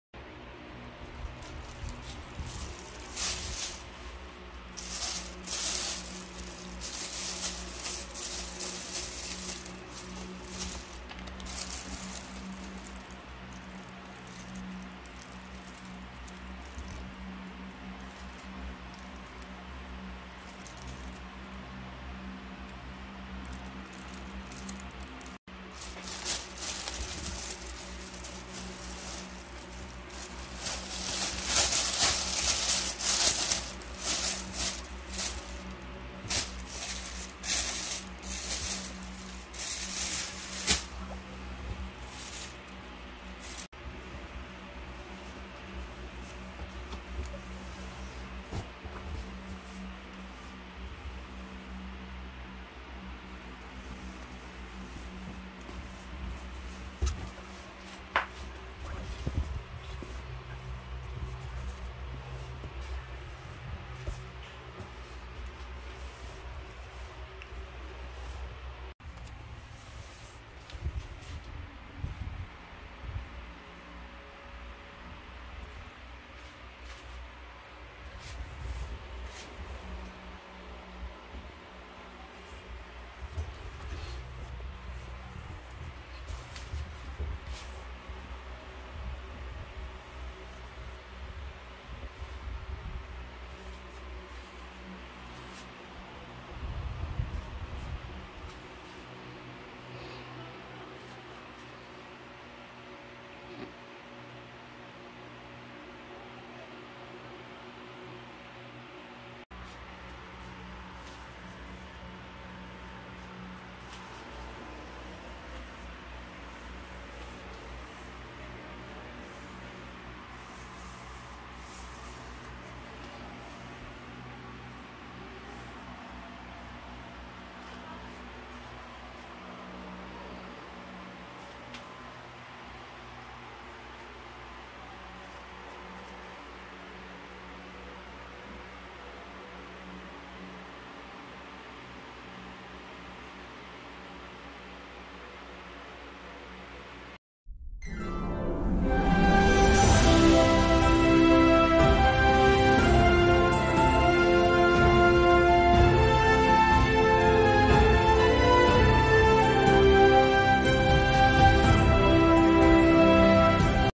My New Philippine Flag (3x6) cotton!
This is my latest collection of Philippine Flag. It is 3 feet by 6 feet made in cotton from "Flag of all Nations" formerly Global Flag Company. 🇵🇭🇵🇭🇵🇭